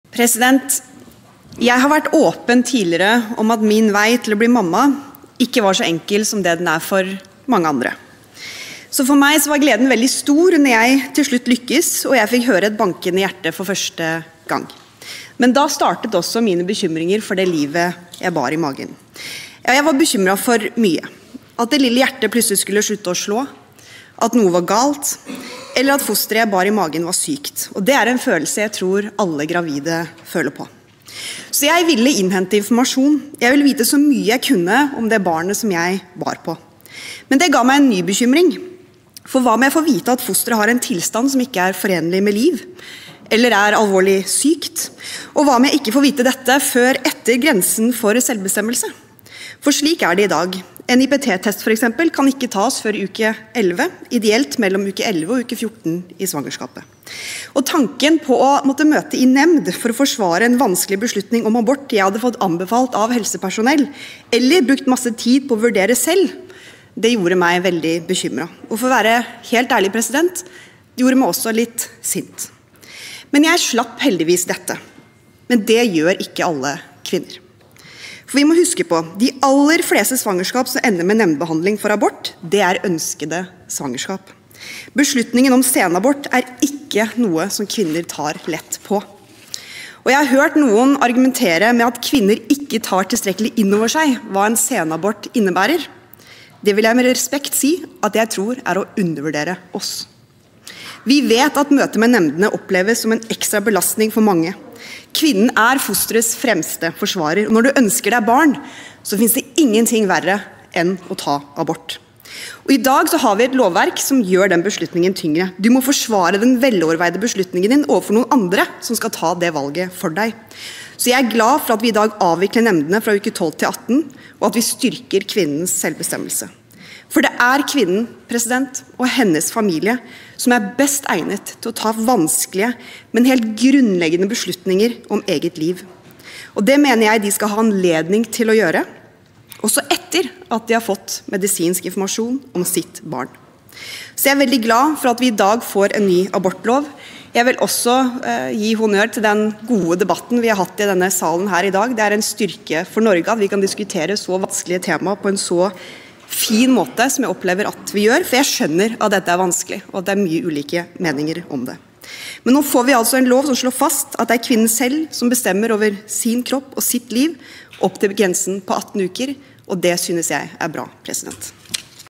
03.12.2024 Sak 4 7 Tina Bru
Om Lov om abort (abortloven) (Innst. 60 L (2024–2025), jf. Prop. 117 L (2023–2024))